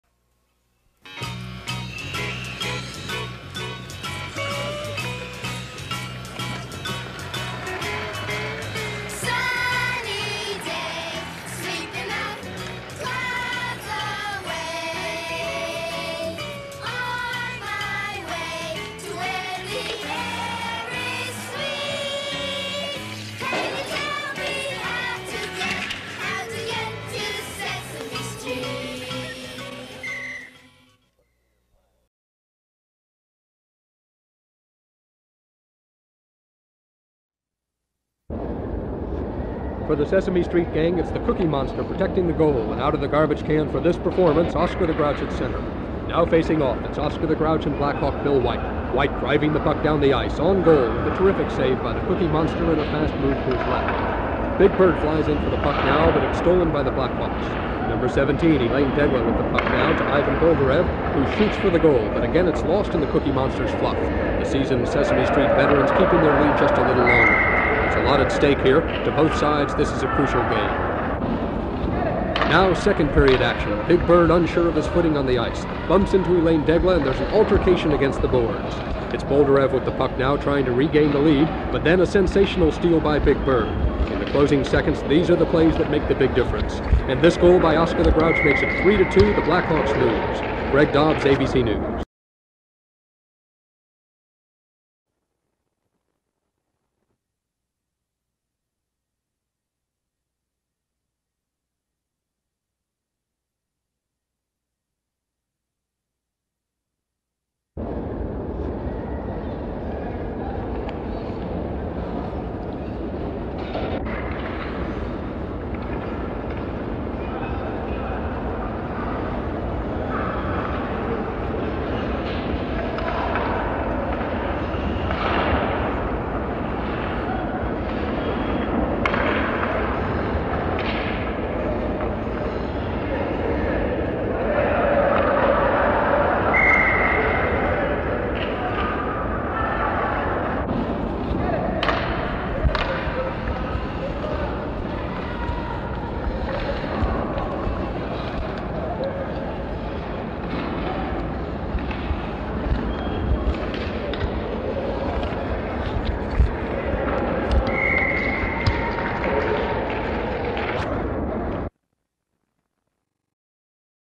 Sesame Street VS Chicago Blackhawks Hockey Game 1975 (Lost Media)
On December 21, 1975, the Chicago Blackhawks went against Sesame Street characters in a game of hockey at the Chicago Stadium for a Christmas Party. In which mascot costumes were used which were the same ones from the Ice Folies ice show that ran in 1974 to 1979. For decades no footage of the event was available online until on October 18, 2011, YouTube channel The Museum of Classic Chicago Television found a partial broadcast of the hockey game itself and 32 seconds of footage of the event of Sesame Street characters. Four years later on June 16, 2015, youtube channel Chicago film archives found more footage of the event from a 16mm reel in SD Quality and it went viral from hockey fans. Nearly five years later the channel uploaded it on YouTube and their website now in HD and the audio of it was found which the clip was actually sourced from a ABC News broadcast because it features a spokesman which the company restored on the YouTube upload while the upload on the website has the clip without the spokesperson audio. On January 7, 2019, the official Chicago Blackhawks account posted a never before seen photo of Ernie with a hockey player and two kids on Twitter and Facebook. To this day, no other available material such as advertisement, newspapers, info, etc of the Seaame Street characters at the hockey game has ever surfaced and only two surviving clips of the event is available online which the rest of the hockey game that featured the Sesame Street characters is lost. Only time will tell if any more material from the event will ever see the light of day again, but I guess we’ll have to wait and see.